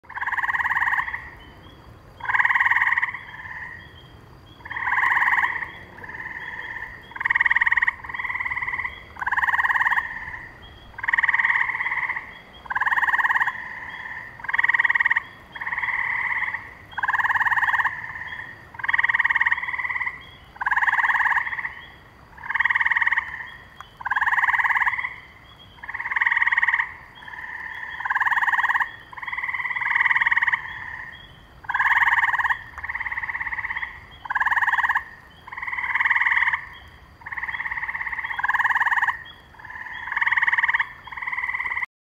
Bullfrog
A big frog stayed still long enough to let me film it a little